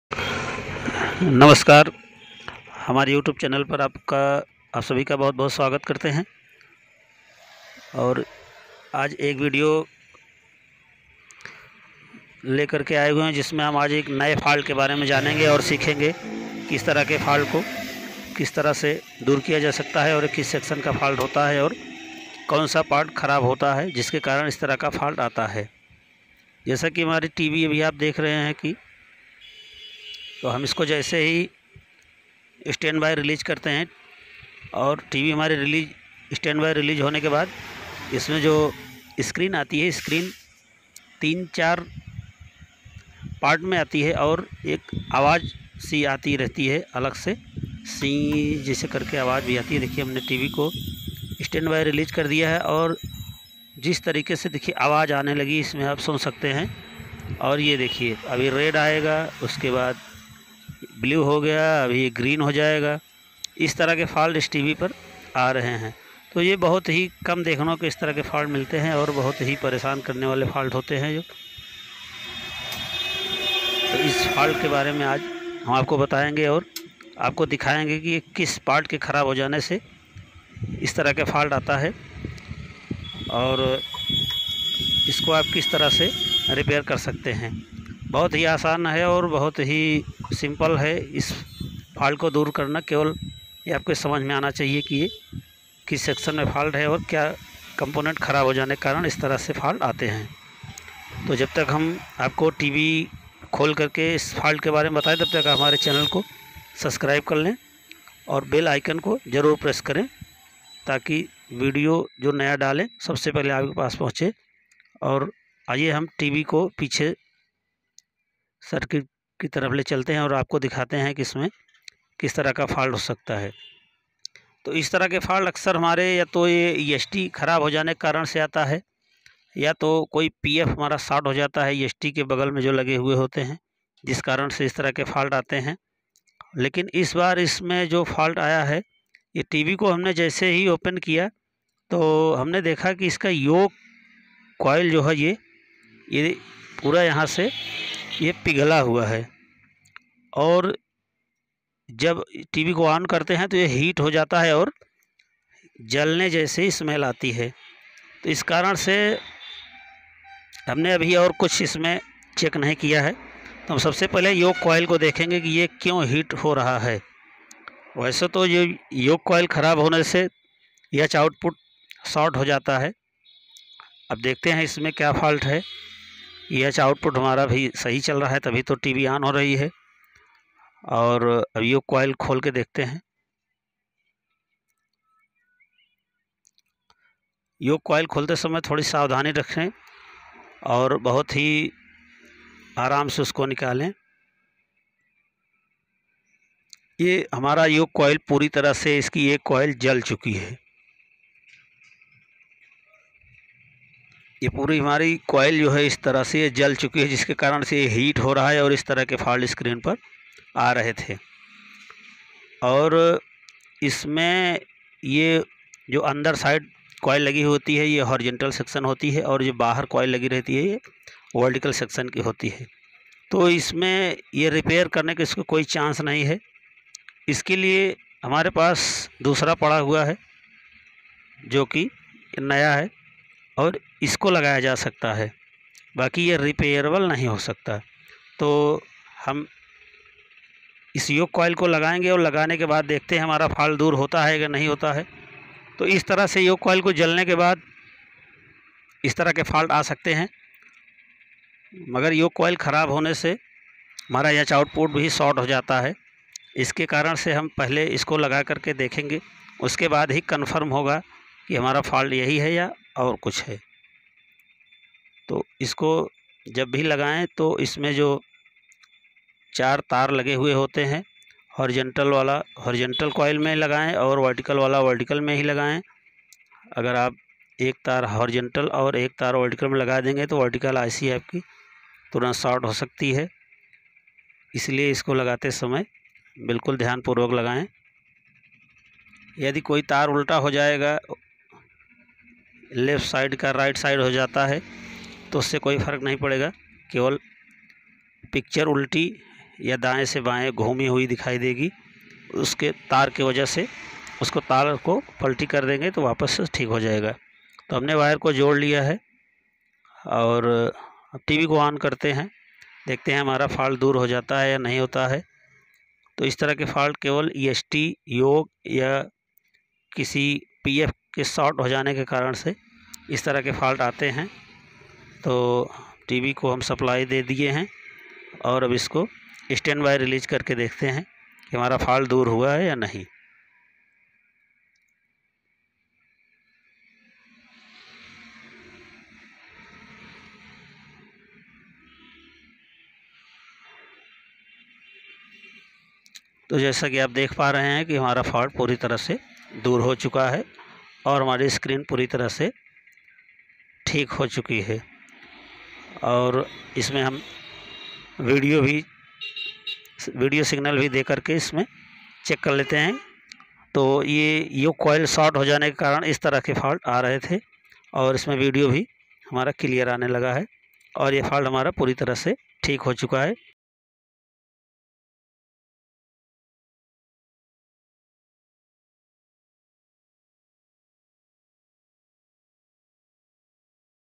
Sansui crt tv sync with AFC problem and no clear screen
Sansui CRT TV AFC problem Singh problem

horizontal problem no clear screen problem